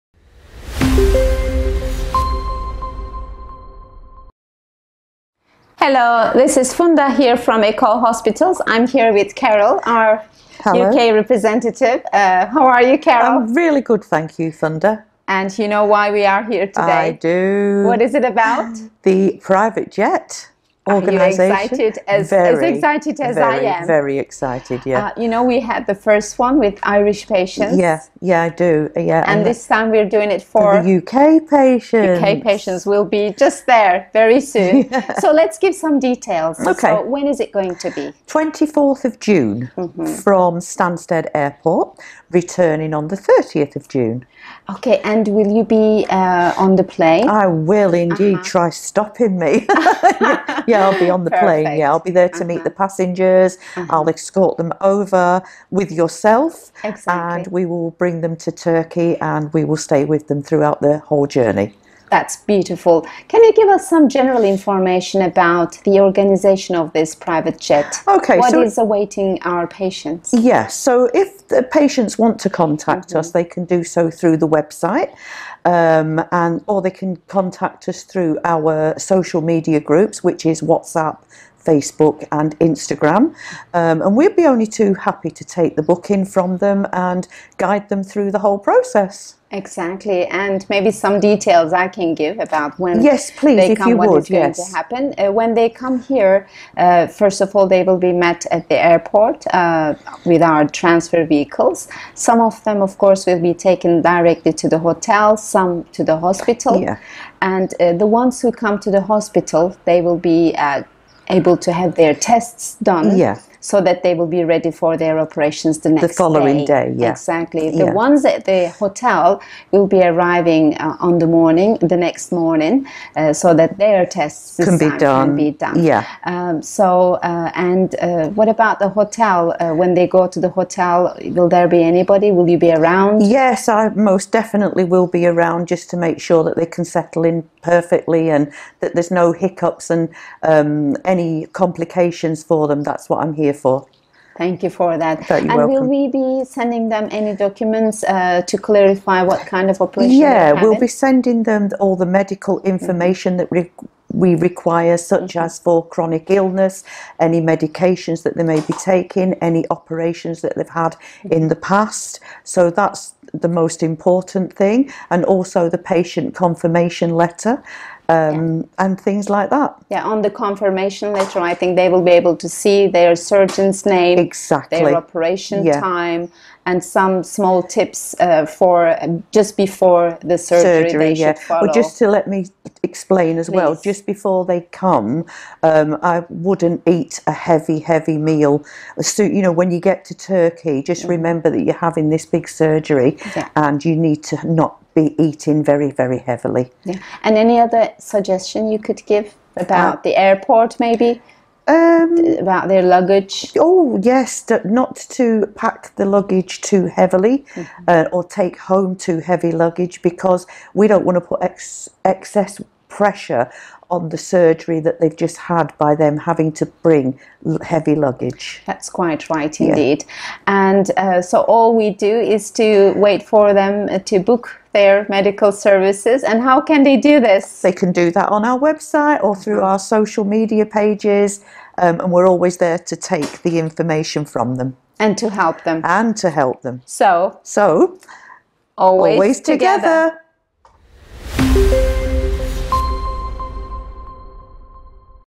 Private Jet Project of Ekol Hospitals
We are flying over to England this time to bring our patients on a Private Jet. Watch this video to get more information about the 2nd Private Jet Project of Ekol Hospitals.